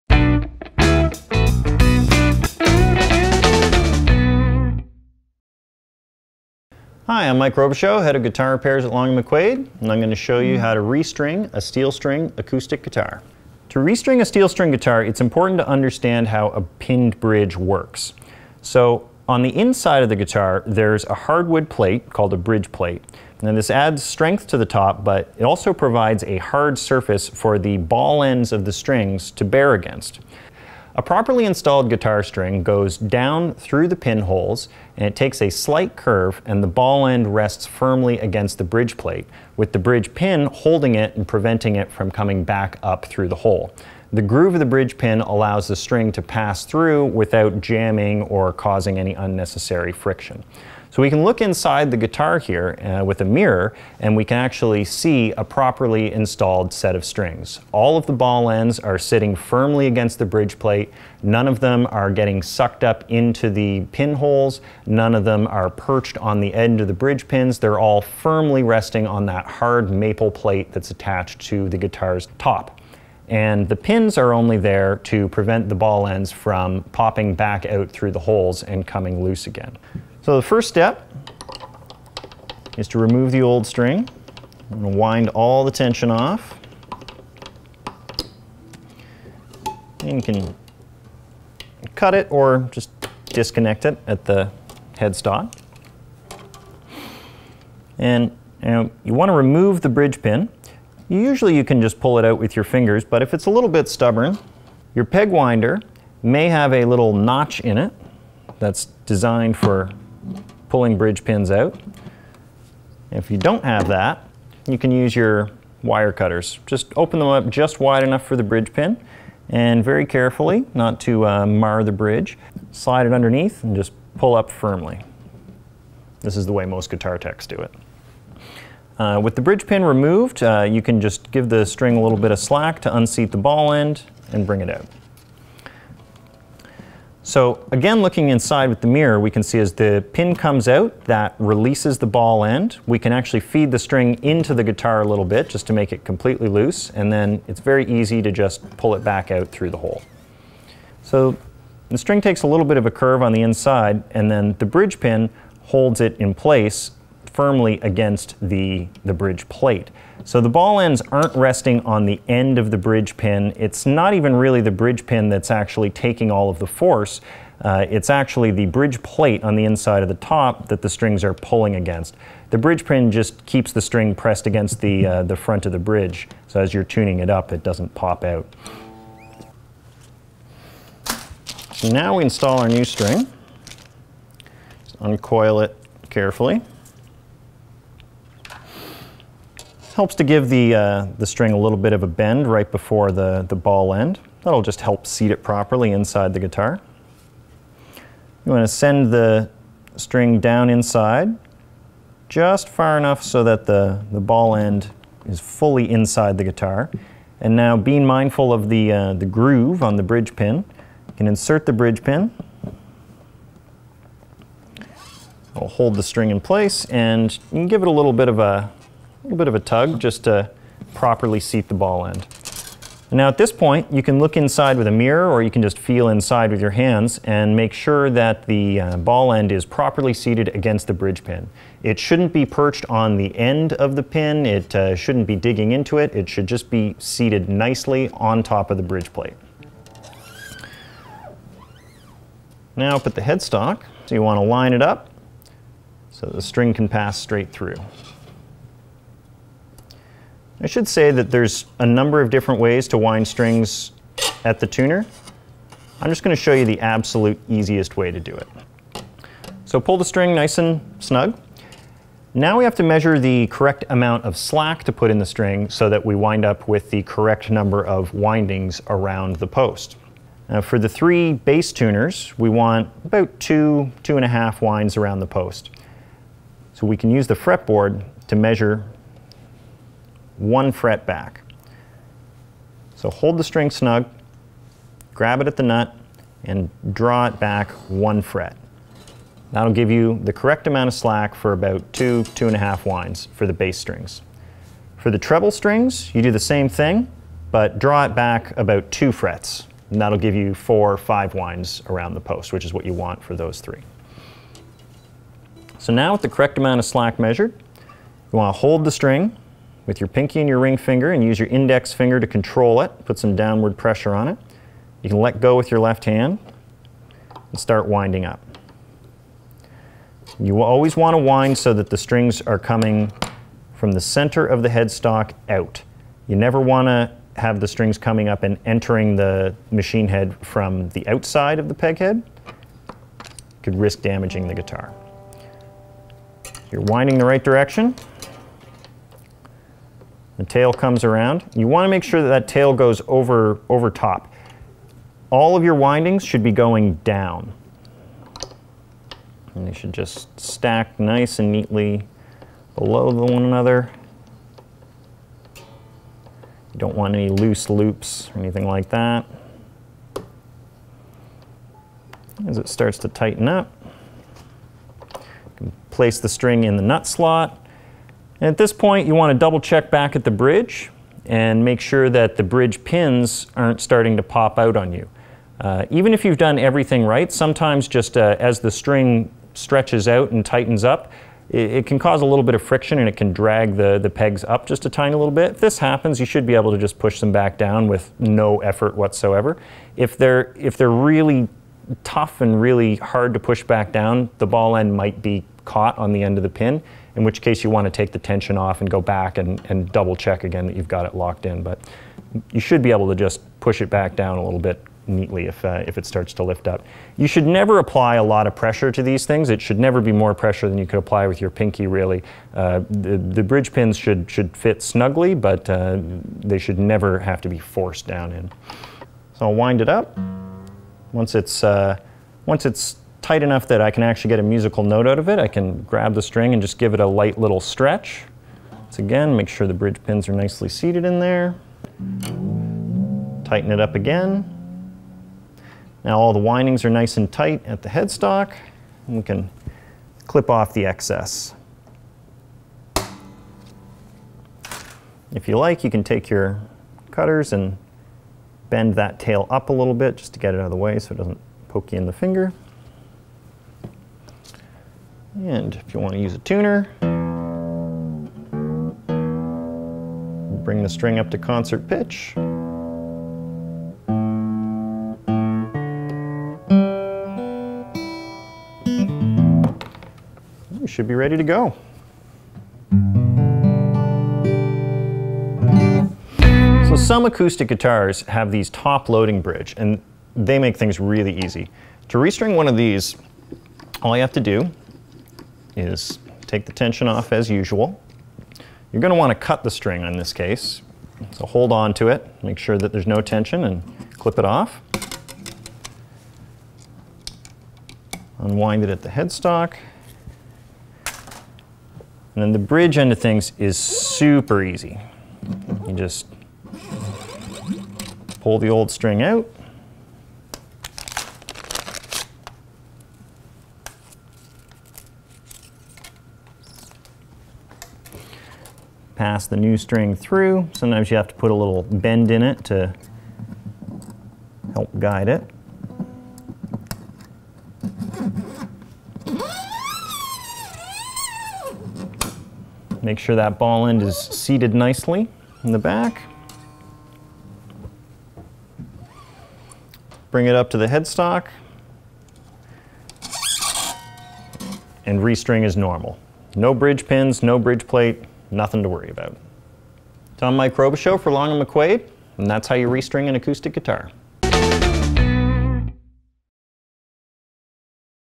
How to Change a Steel String on an Acoustic Guitar
Here's the quickest and easiest way to properly replace an old or broken guitar string on a typical steel string acoustic guitar.

For more restring instructional videos on other guitar models, check out our How to Restring Your Instrument playlist,